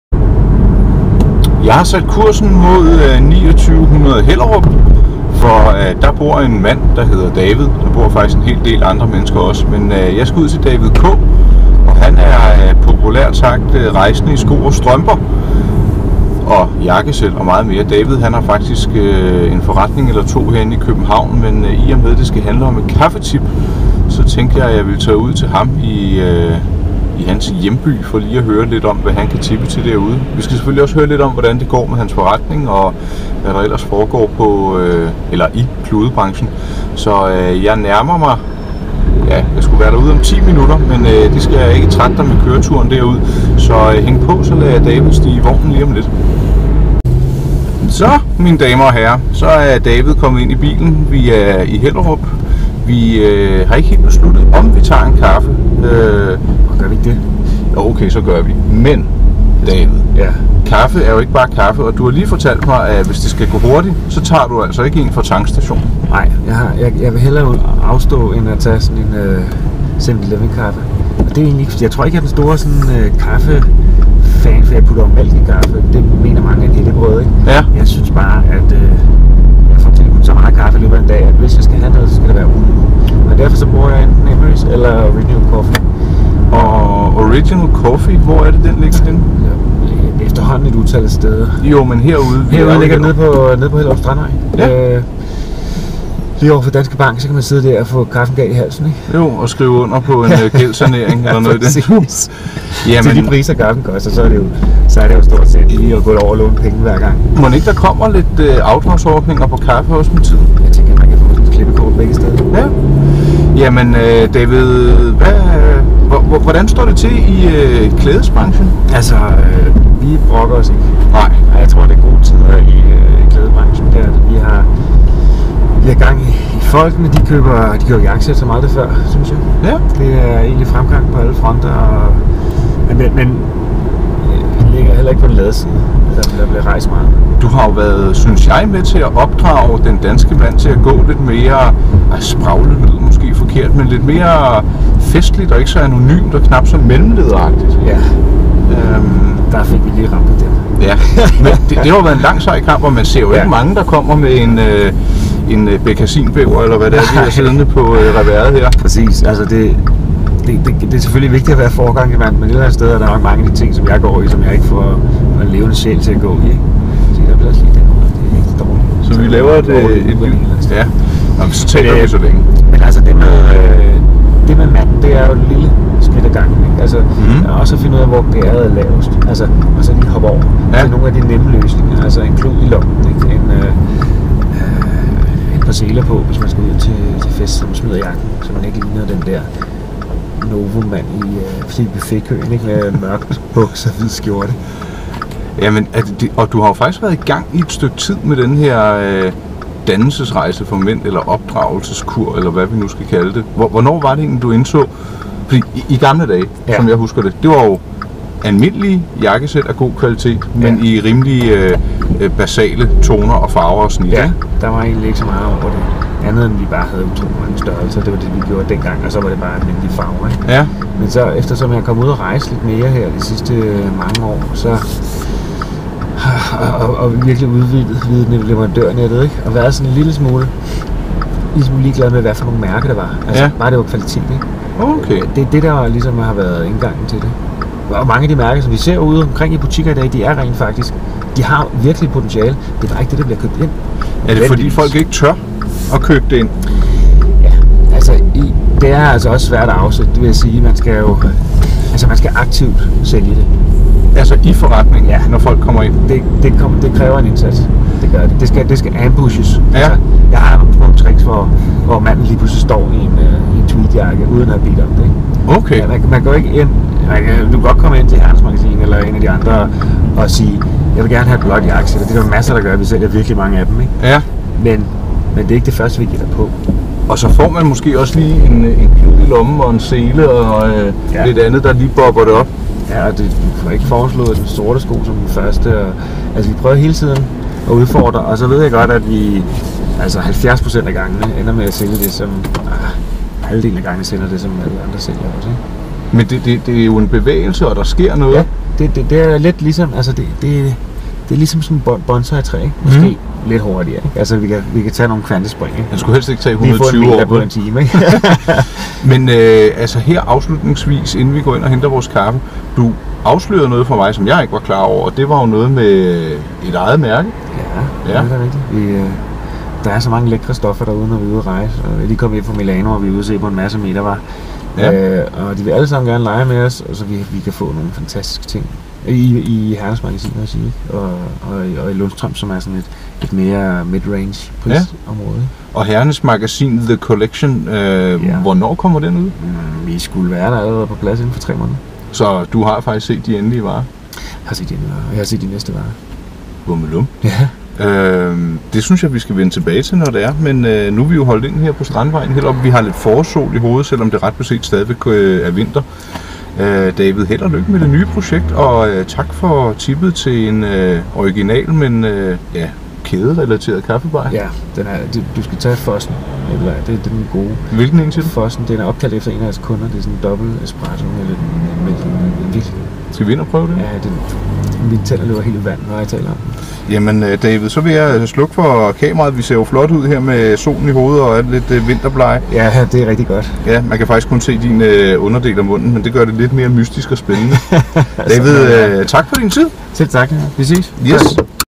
My-Pleasure.dk & VW Beetle præsenterer: Kaffetip med David K
Mød David Krarup. Han tipper i denne video til kaffe i 2900 Hellerup, men vi kommer også omkring branchen for herre-ekvipering...